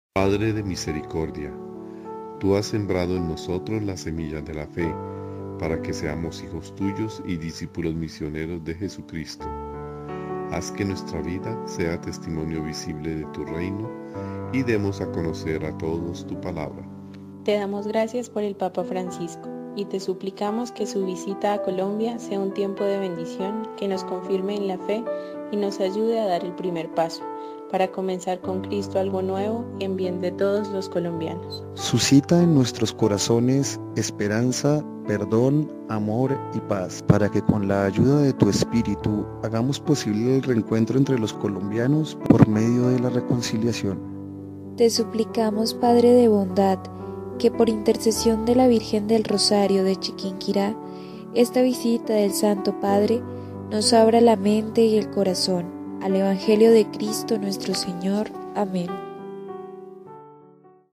ORACIÓN POR LA VISITA DEL PAPA FRANCISCO A COLOMBIA 2017
Padre de misericordia,
Tú has sembrado en nosotros la semilla de la fe,
para que seamos hijos tuyos y discípulos misioneros de Jesucristo.
Haz que nuestra vida sea testimonio visible de tu Reino
y demos a conocer a todos tu Palabra.
Te damos gracias por el Papa Francisco,
y te suplicamos que su visita a Colombia
sea un tiempo de bendición,
que nos confirme en la fe
y nos ayude a dar el primer paso,
para comenzar con Cristo algo nuevo
en bien de todos los colombianos.
Suscita en nuestros corazones
esperanza, perdón, amor y paz,
para que con la ayuda de tu Espíritu
hagamos posible el reencuentro entre los colombianos
por medio de la reconciliación.
Te suplicamos, Padre de bondad,
que, por intercesión de la Virgen del Rosario de Chiquinquirá,
esta visita del Santo Padre,
nos abra la mente y el corazón
al Evangelio de Cristo nuestro Señor.
Amén.